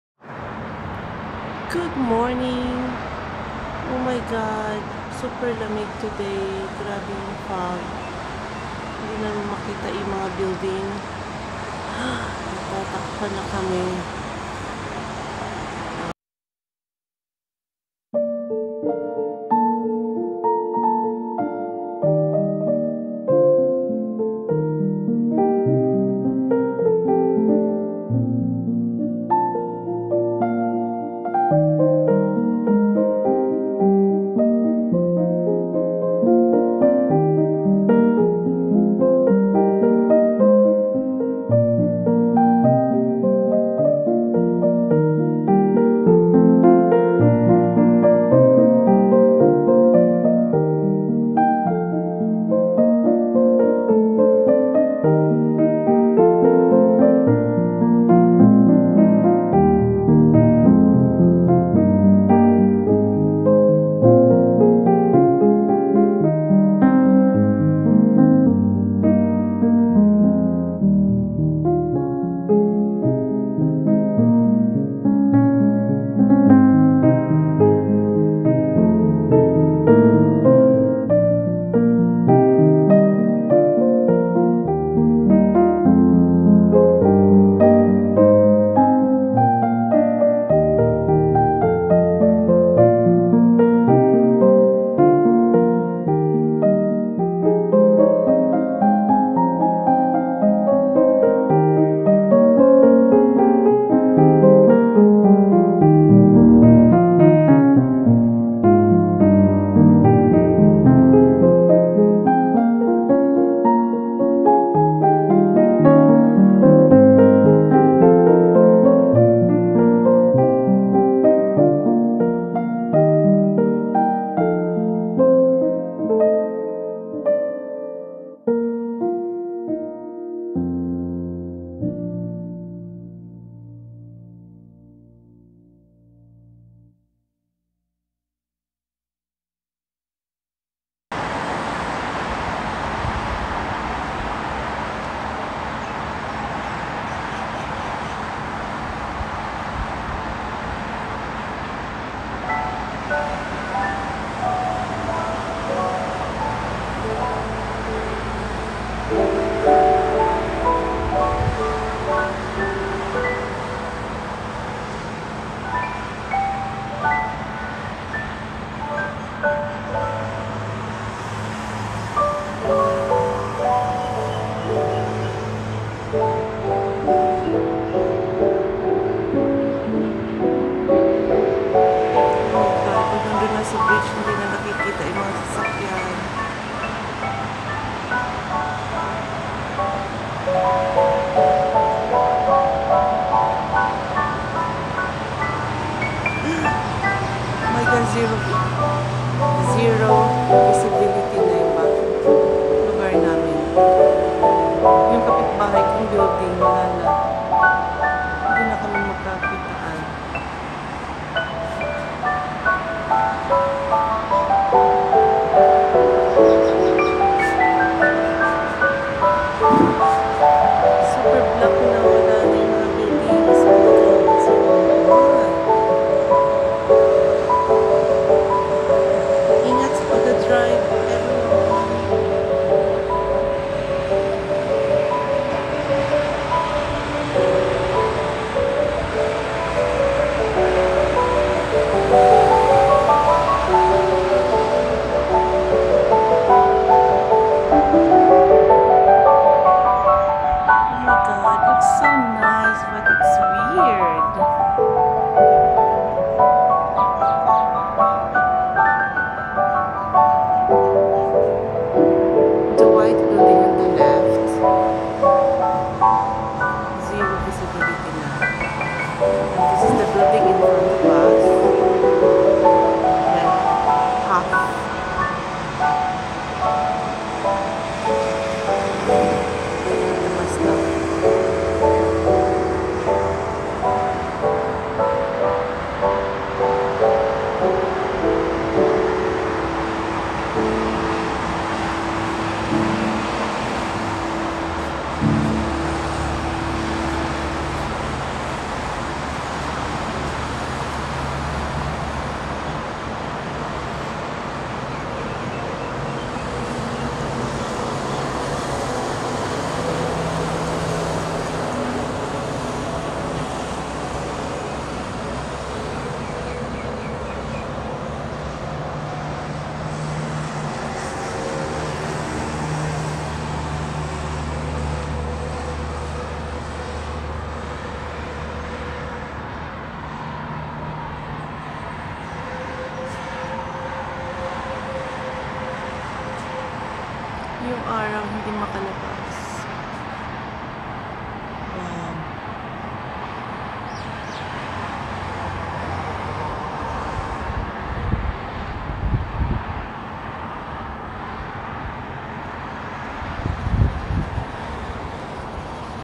THE BUILDINGS ARE GONE:FOGGY MORNING
SOMETIMES WHEN THE WEATHER IS COOLER ,YOU WILL EXPERIENCE A FOOGY MORNING IN DUBAI UAE, YOU CANT IMAGINE TO SEE THE BEAUTY OF DUBAI BUILDINGS SOUROUNDED WITH FOD WHEN YOU WAKE UP ,LOOKS LIKE THE CLOUDS ARE JUST BESIDE YOU.